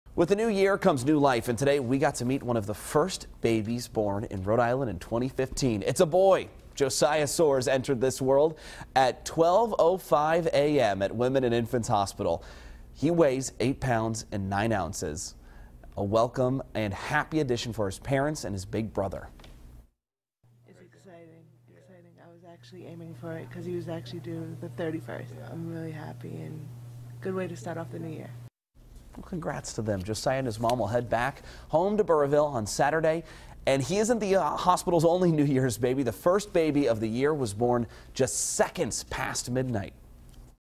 Women & Infants Hospital Welcomes New Year’s Newborns
New Year’s Day is viewed by many as a time for new beginnings, and that’s especially true for families bringing new life into the world.